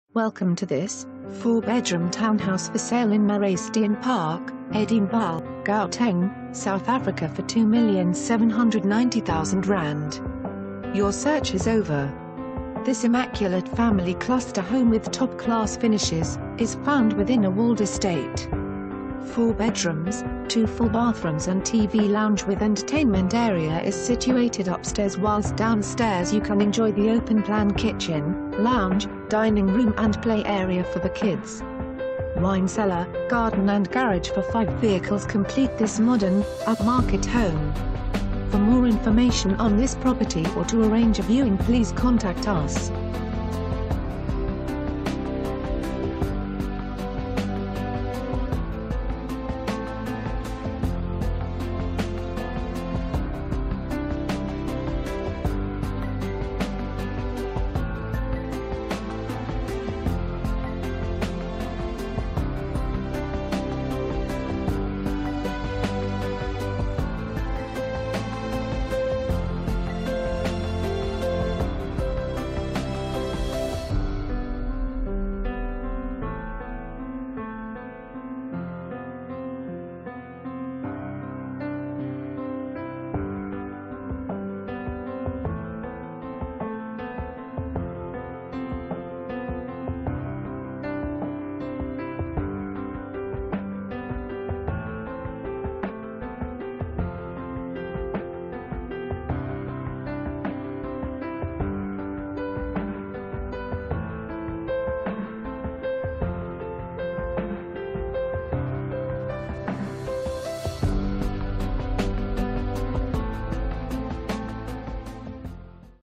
4 Bedroom Townhouse For Sale in Marais Steyn Park, Edenvale, Gauteng, South Africa for ZAR 2,790,000
Your search is over! This immaculate family cluster home with top class finishes, is found within a walled Estate. Four bedrooms, two full bathrooms and TV Lounge with entertainment area is situated upstairs whilst downstairs you can enjoy the open plan kitchen, lounge, dining room and play area for the kids.

Wine cellar, ...